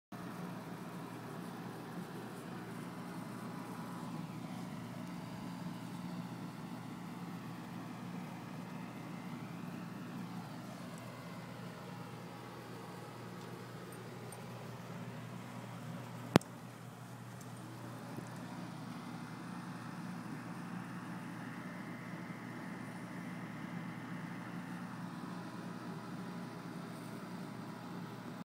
Snowy Morning in Dayton
I woke up this morning with 2-3" of snow on the ground in Dayton, OH.